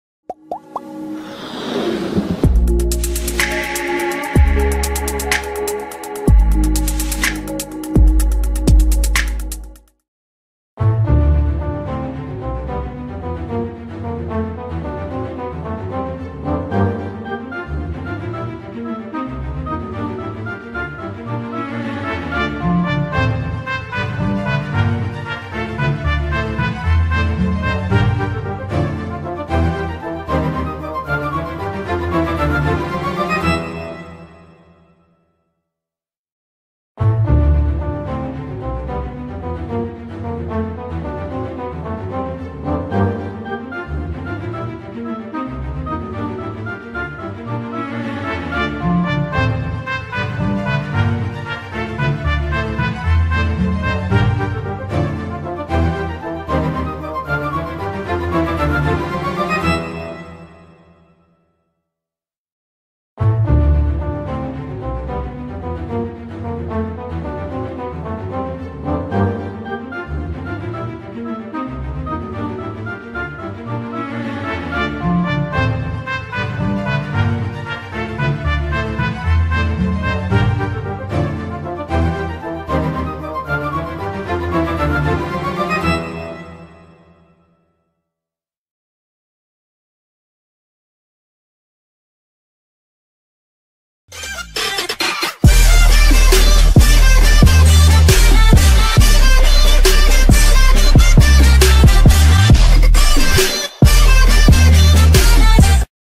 Les Inno­cents : Qui est Jules Houplain, le jeune acteur qui incarne Yann Desgrange ?
Jules Houplain joue dans la série Les Inno­cents, dont le dernier épisode est diffusé jeudi 25 janvier, à 20h55 sur TF1. Il y incarne un adoles­cent ,Yann, témoin clé d'une tuerie. À 18 ans, le jeune ... ...